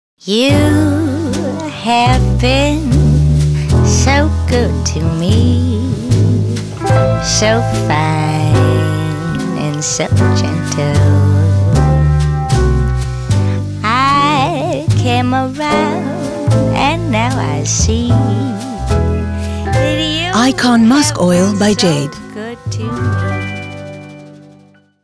ICON MUSK OIL - הבושם המומלץ אייקון מאסק אויל מבית ג'ייד
אייקון מאסק אויל - ICON MUSK OIL מבית ג'ייד.
תווי הניחוח של הבושם לאשה מאסק אויל נפתחים בזוהר של זר פרחים לבנים, חמימים וענוגים, הכוללים את שושנת העמקים, ורד קטיפתי ויסמין פורח ומהווים מוטיב מרכזי בניחוח המרענן. תמצית פרחי מאסק לבנים וצלולים משתלבת איתם בהרמוניה ומשלימה את הניחוח החושני.

Dorit Revelis for ICON MUSK OIL by GA-DE
The pure fragrance of endless seduction.
Director & Creative : Simon Elmalem